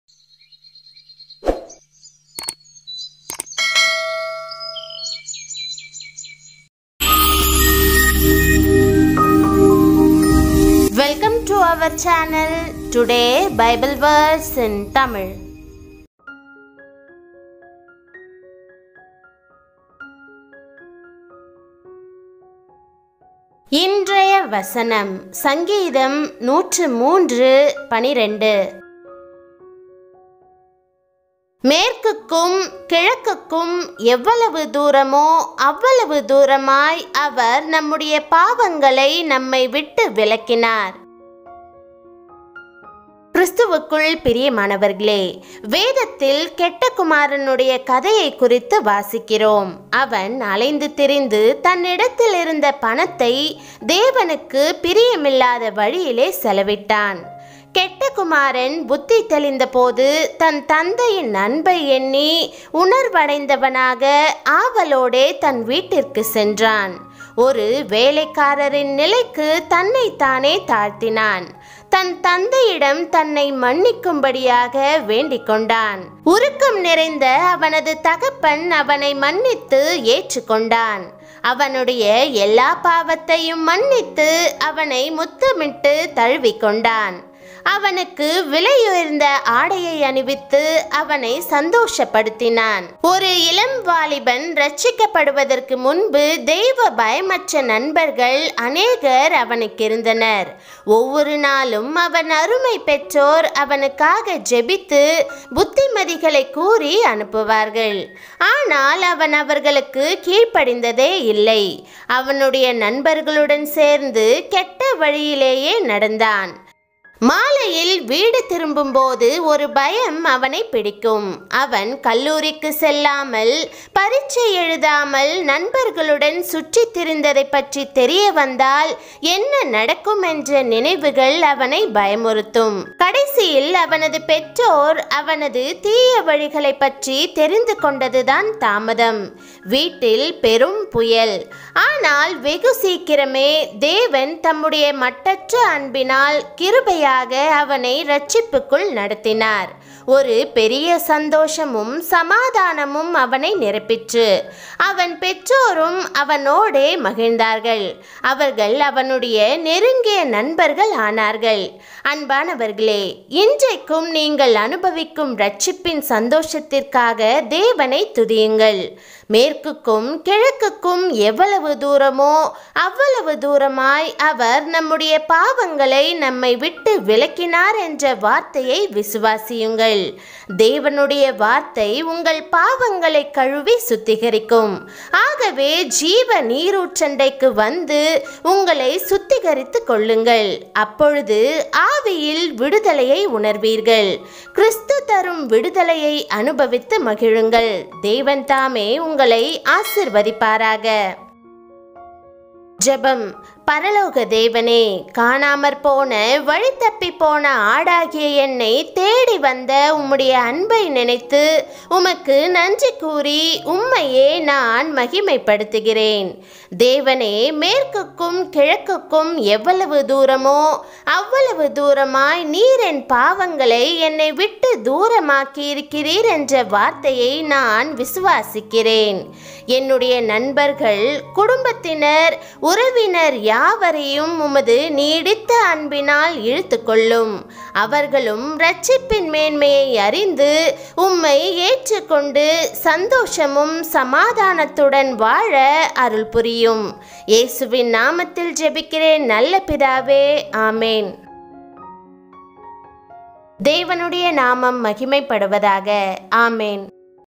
Today Bible Verse in Tamil I Today Bible Verse I Today's Bible Verse I Bible Verse Today I24.06.2021
ENGLISH VERSION: Today's Bible Verse (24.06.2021):

VERSE:
As far as the east is from the west, so far hath he removed our transgressions from us. (PSALM 103:12 KJV)

EXPLANATION:
In the scriptures we read about the story of the bad son.  He wandered and spent the money he had in a way that was not pleasing to God.

The bad son went to his house eagerly, realizing his father's love when he came to his senses.  He degraded himself to the status of a servant.  He begged his father to forgive him.

His heartbroken father forgave him and accepted.  He forgave all his sins and kissed and embraced him.  He dressed him in expensive clothes and made him happy.

Before a young man was saved, he had many godly friends.  Every day his nobles would pray for him and send him advice.  But he never obeyed them.  He went in a wrong way with his friends.

When he returns home in the evening a fear catches him.  Memories of what would happen if he found out about wandering around with friends without going to college and writing the exam scared him.

In the end, it was only a matter of time before his parents found out about his evil ways and there was a great storm at home.  But soon God, by His infinite love, graciously led him into salvation.  A great joy and peace filled him.  His parents enjoyed him too.  They became his close friends.

Beloved, praise God for the joy of salvation that you enjoy today.  Believe in the saying, "As far as the west and the east are from the heavens, so far off from us he has put our sins."

The Word of God will wash away your sins.  So come to the fountain of life and purify yourself.  Then you will feel liberated in spirit.  Enjoy the deliverance that Christ gives.  God bless you!

PRAYER:
I glorify Thee, O God of heaven, for thy loving kindness which came to me in the lost way.  O God, I believe the word, that as far as the west and the east are, thou hast put my sins far from me.  My friends, family and relatives will be drawn by your lasting love.  They too will know the superiority of salvation and accept you and bless you to live in happiness and peace.  I pray in Jesus' name Good Father, Amen.